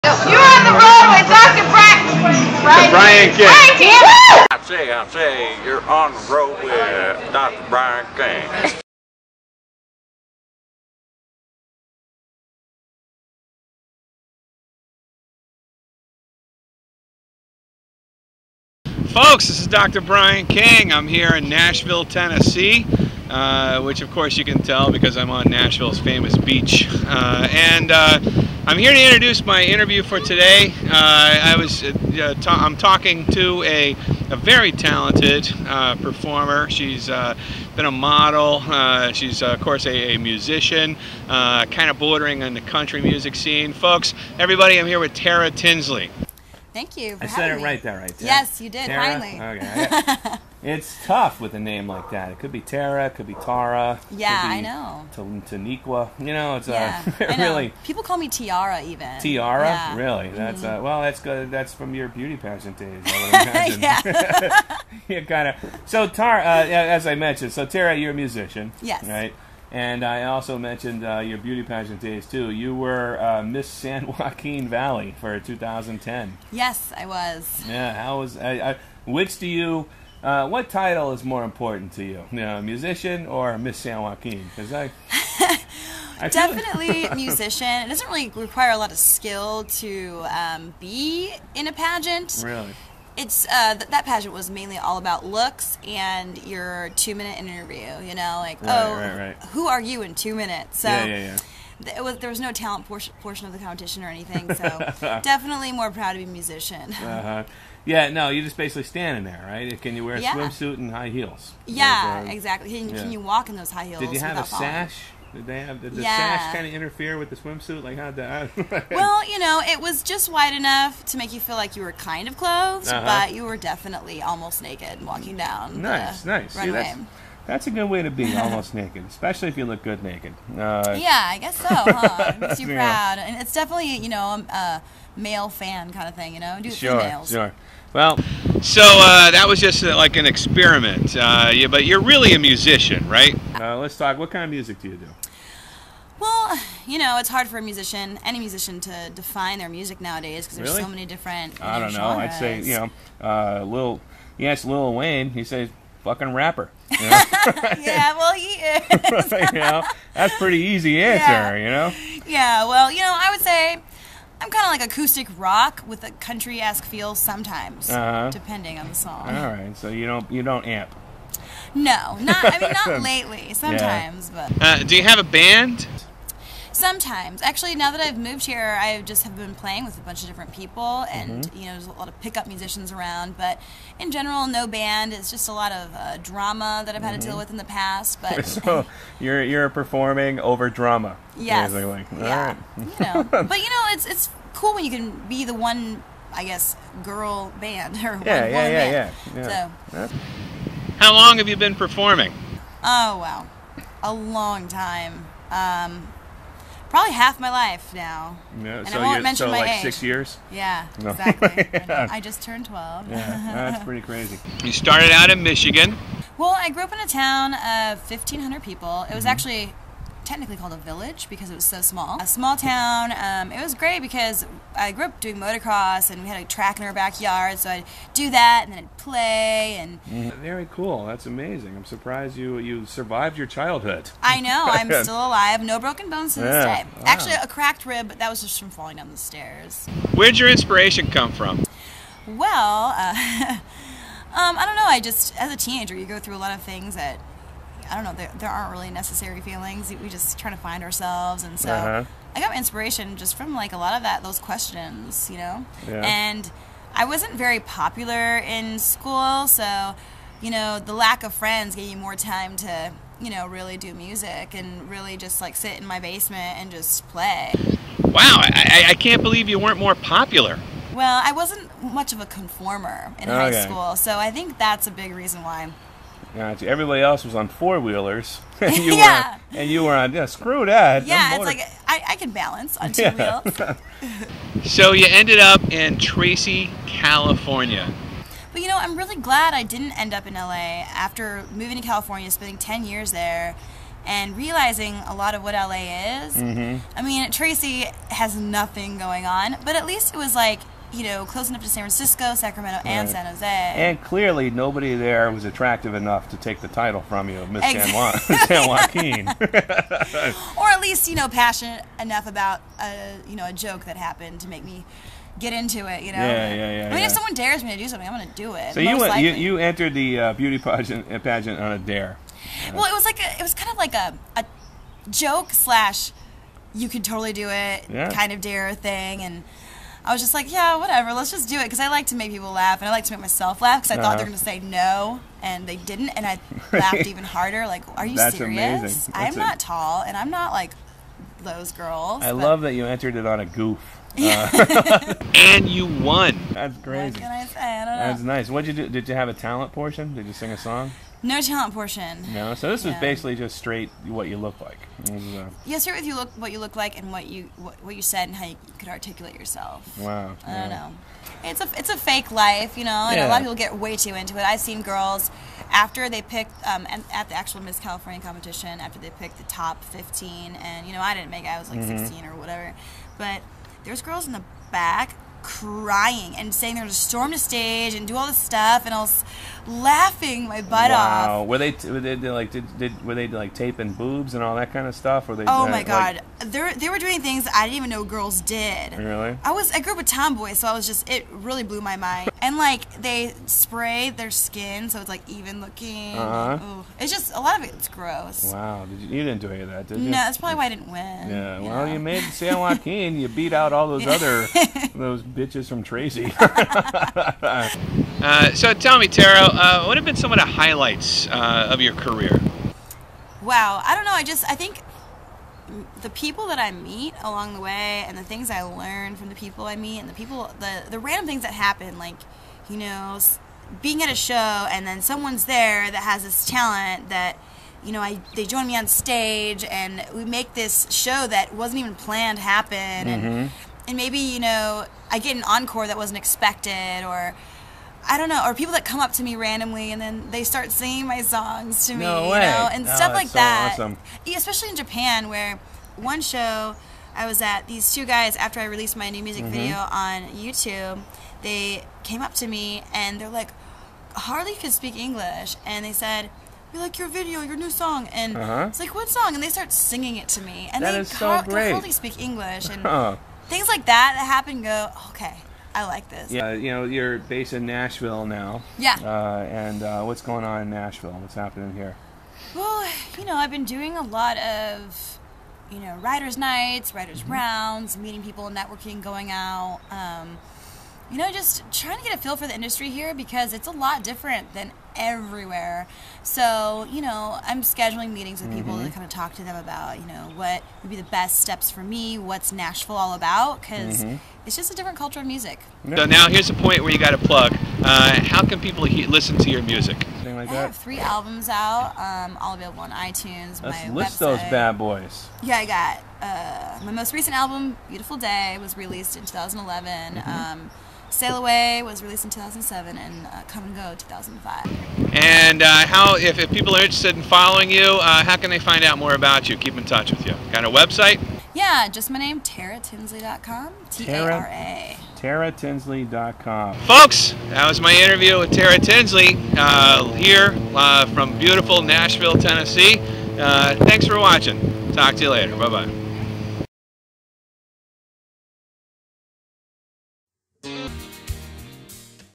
Nashville Recording Artist Tara Tinsley
Hanging out in Nashville Beach, Tennessee Dr Brian interviews recording artist Tara Tinsley